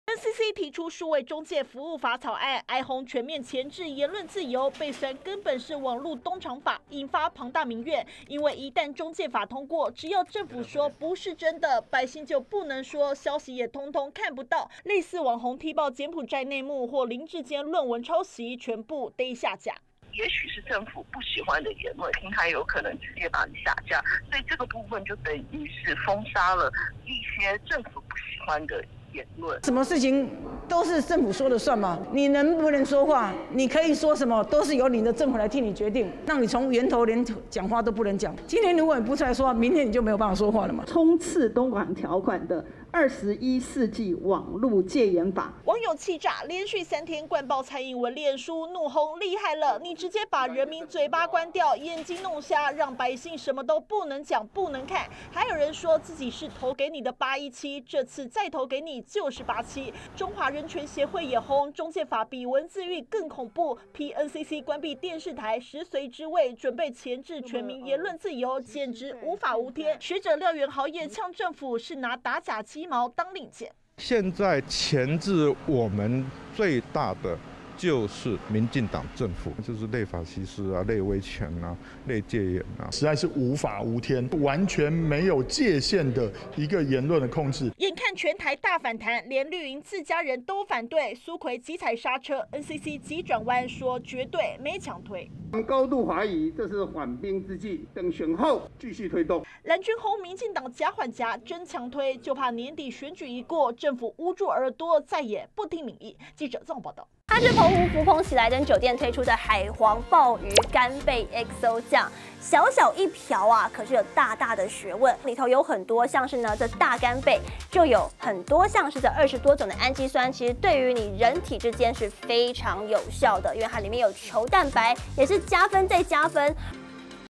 【每日必看】中介法比文字獄恐怖? 網炸鍋嗆蔡:再挺你是87 20220822@中天新聞CtiNews
NCC推數位中介服務法草案，引發各界反彈，網友更連續三天灌爆蔡總統臉書，嗆政府直接把人民嘴巴關掉、眼睛弄瞎。中華人權協會也發聲明，認為中介法比"文字獄"更恐怖，痛批政府箝制全民言論自由，簡直"無法無天"。